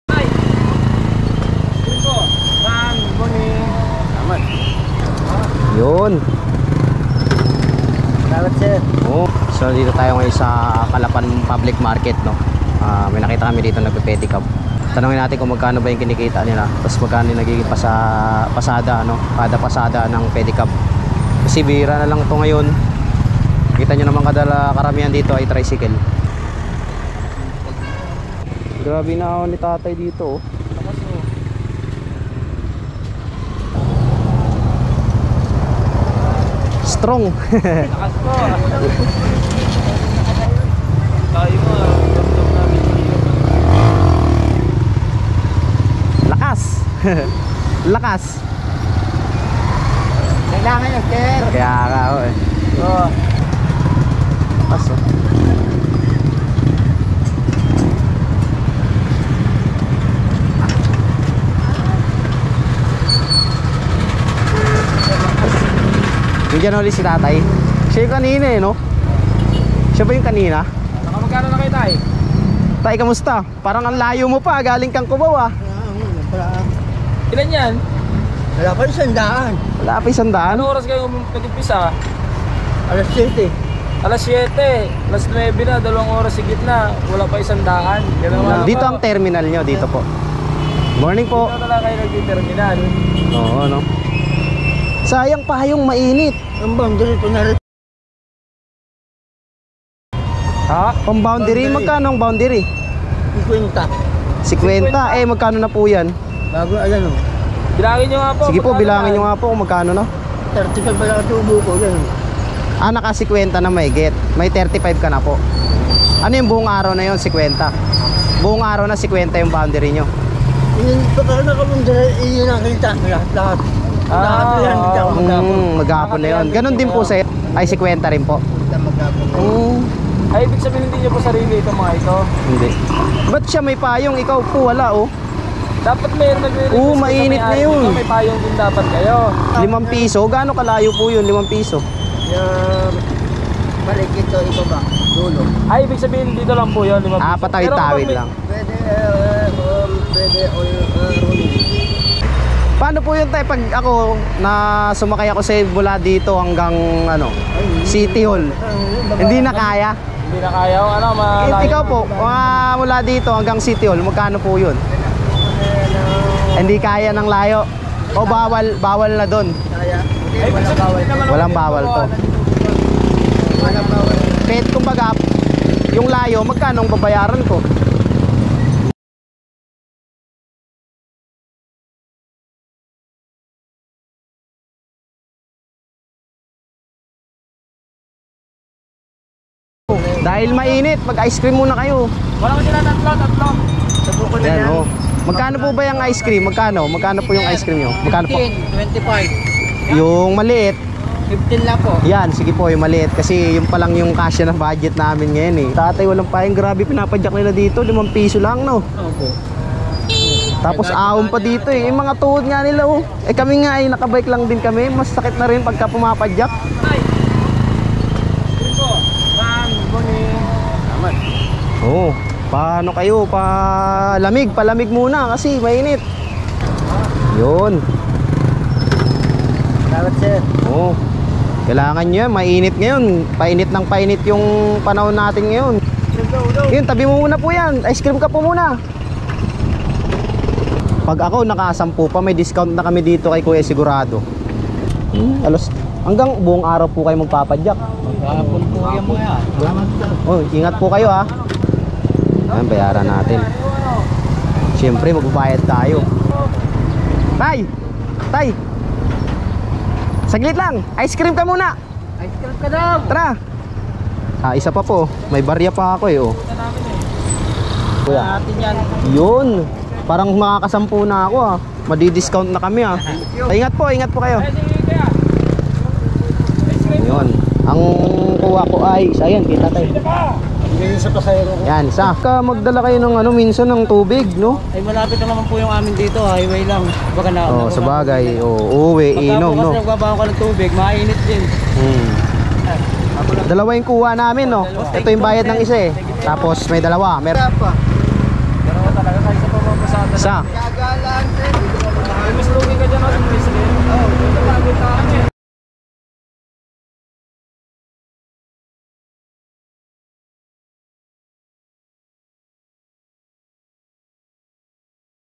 5 pesos na pamasahe sa pedicab ‼️
Sa 5 pesos na pamasahi sa pedicab ang pinagtatyagaan ni tatay na may kapansanan pa ngunit hindi ito hadlang upang syang magtrabaho ng marangal.